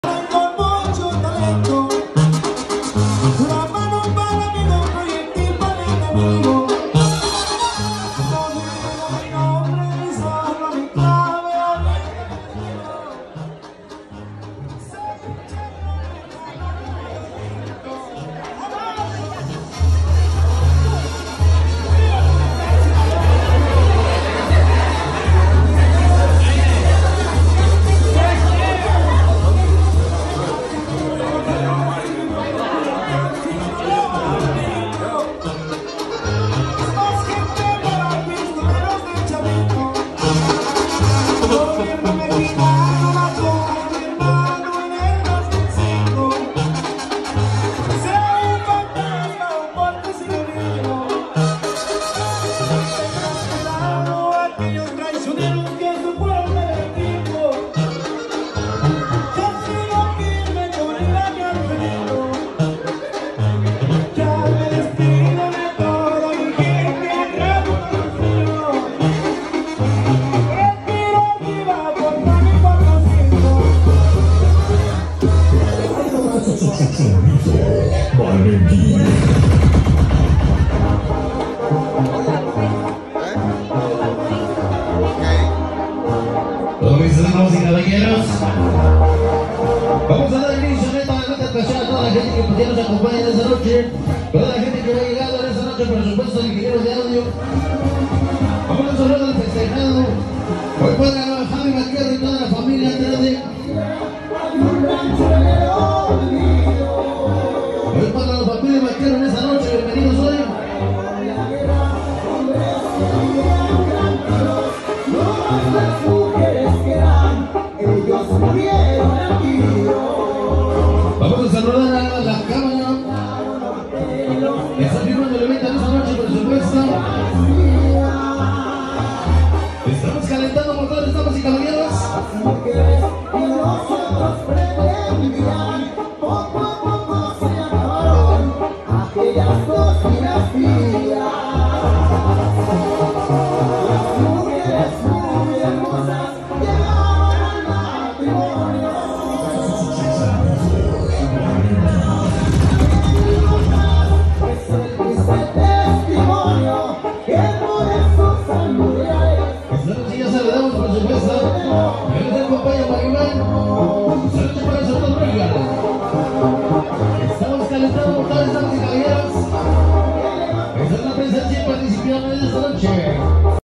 Fiesta privada con Sonido malinky con la familia Vaquero
Audio e iluminación profesional Sonido Royal 98 👌